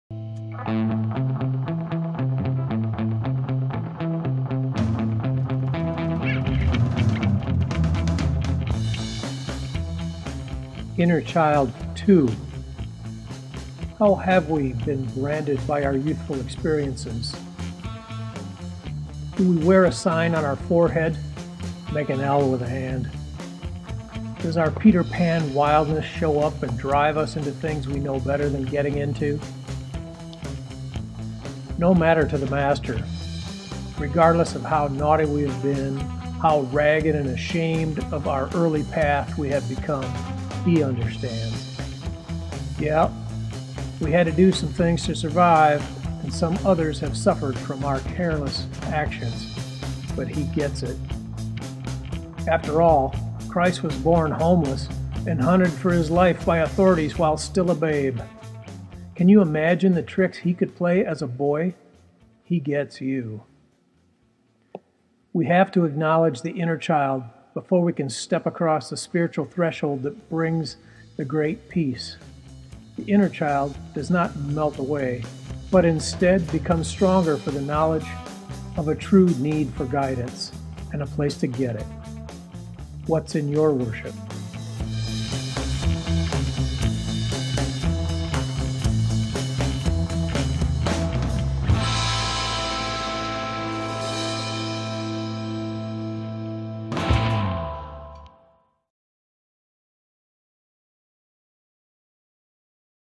Inner Child2 TjA
Relief Sculpture "Inner Child 2" by Tj Aitken described and pictured with close ups of details. An auction item for the "Victory Over Lyme" campaign, a benefit for Chronic Lyme disease patient Medical bills.